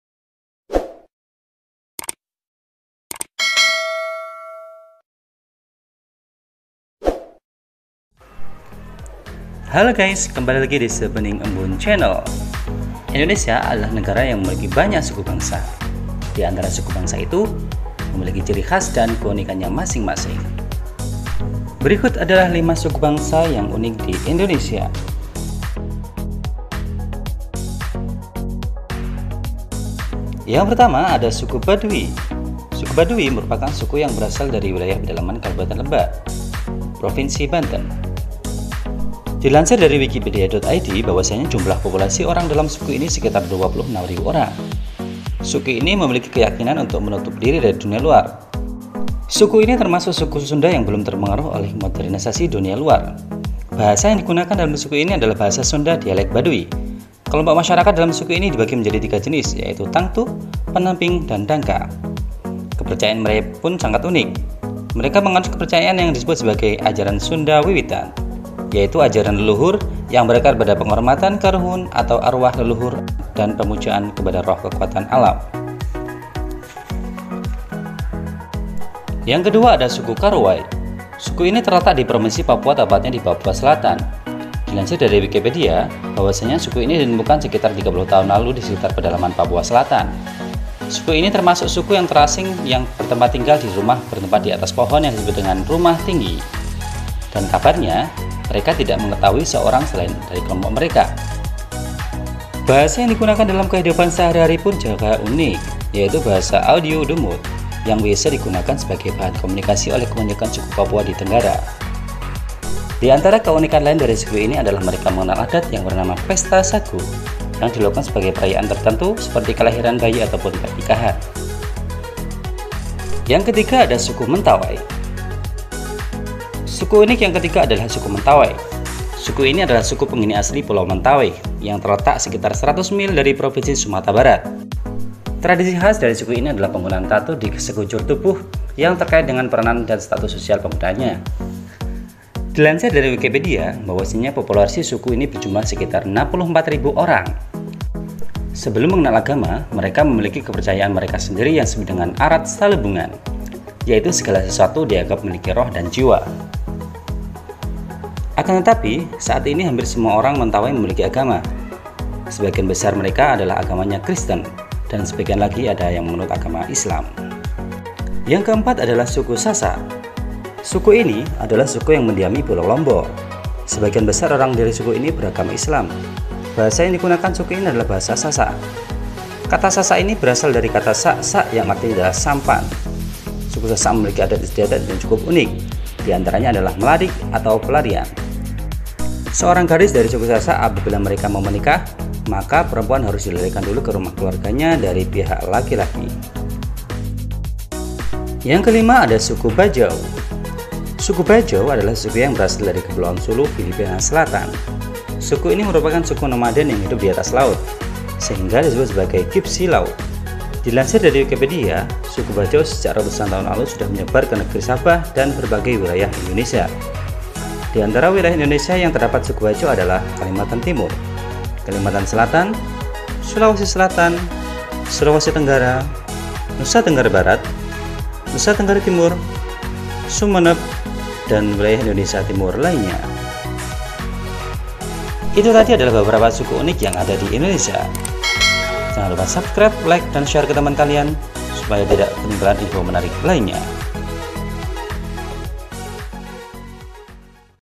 5 SUKU PALING UNIK DI INDONESIA ! INDONESIA PUNYA !
Indonesia adalah negara yang memiliki ribuan suku bangsa yang memiliki keunikan dan ciri khasnya masing masing.